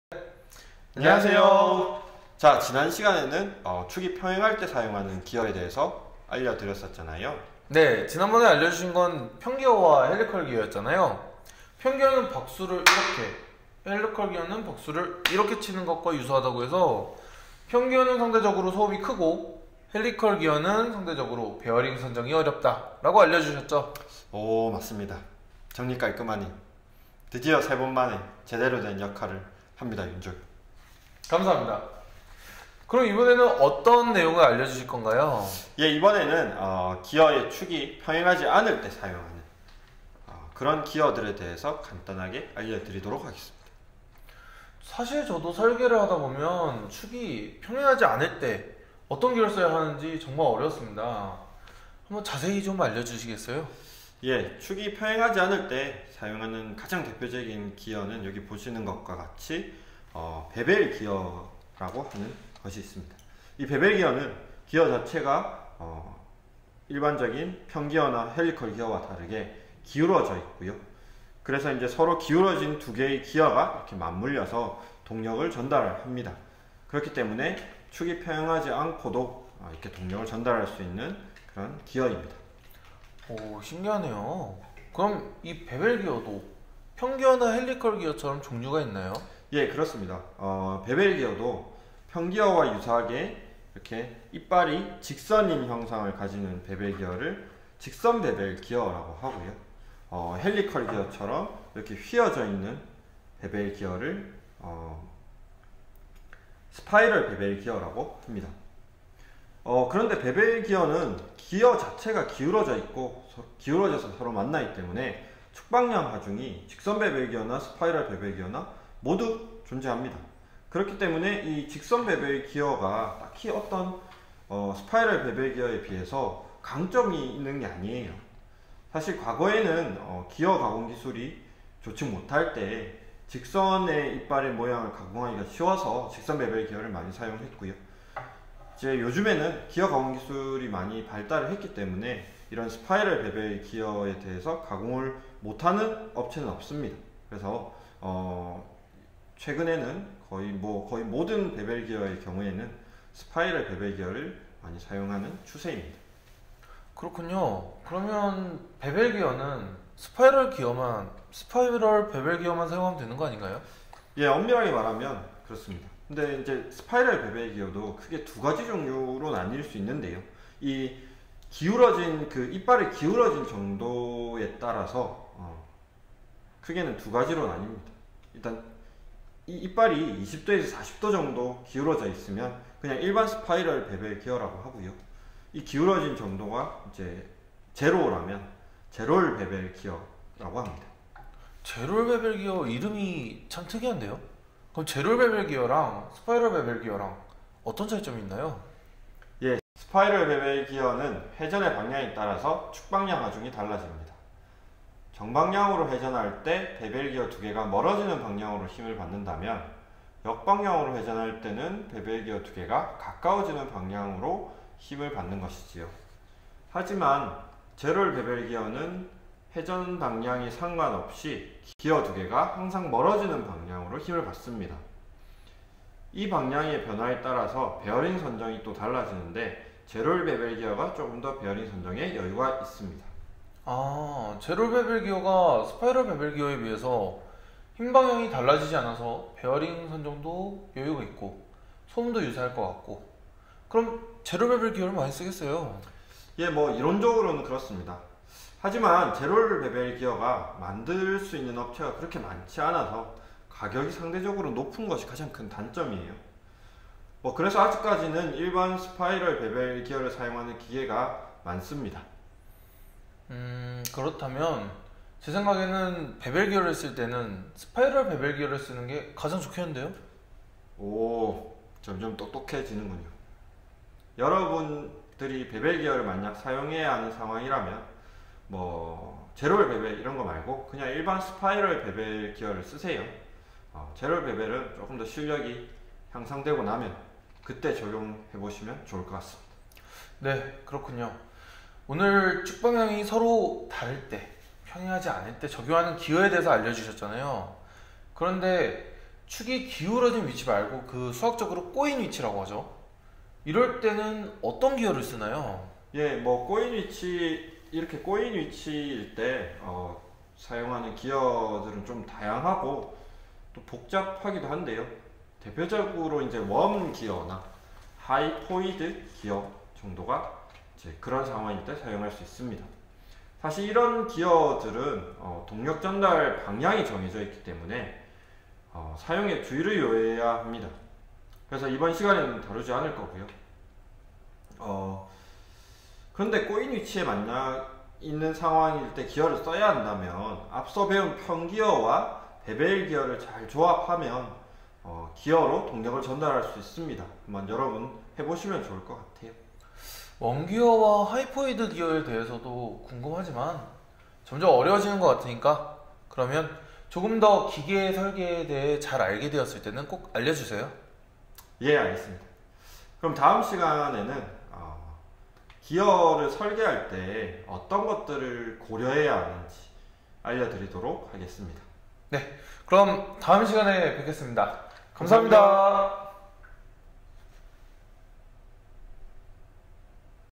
기어설계 초급. Ep02. 축이 평행하지 않을 땐 베벨기어
평기어, 헬리컬기어에 이은 베벨기어
축이 평행하지 않을 땐 스파이럴 베벨기어를 쓰면 무난합니다.^^